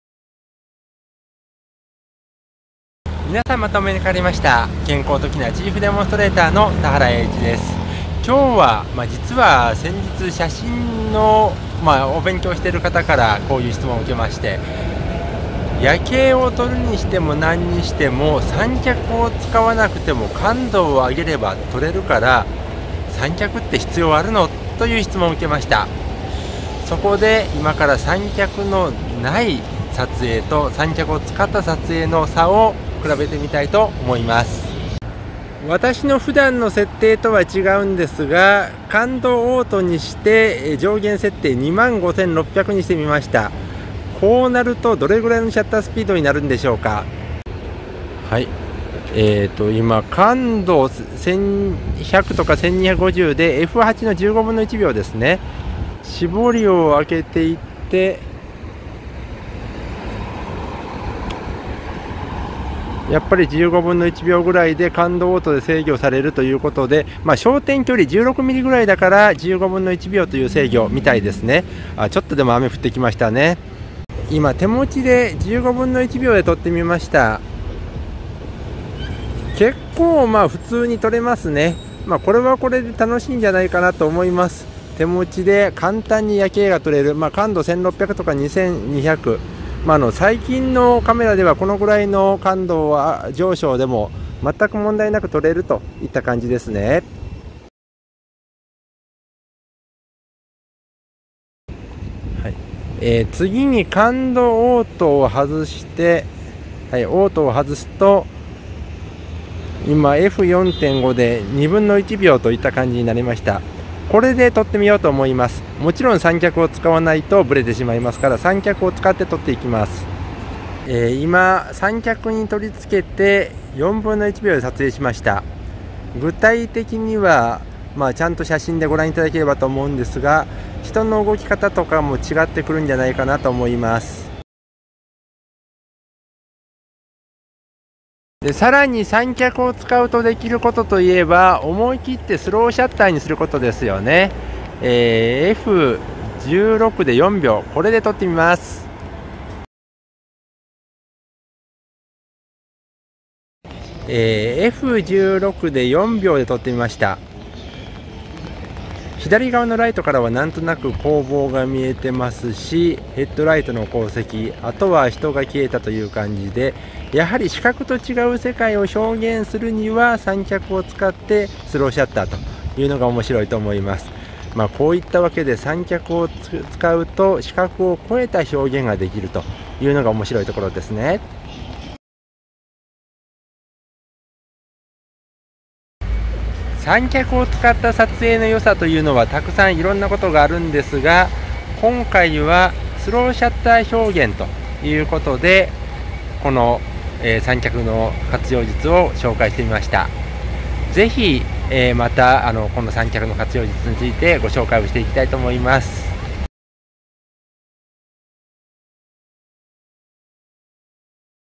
【三脚を使うと】手持ち夜景と三脚での夜景撮影（動画No.393）
「手持ちで夜景を撮れるけど、三脚を使うとどう違うの？」と聞かれて、説明してみました。

(KT) TOKINA トキナー opera(オペラ)シリーズ opera 16-28mm F2.8 FF ニコンFマウント用 ケンコートキナー KENKO TOKINA

SLIK スリック 三脚 システムカーボン 73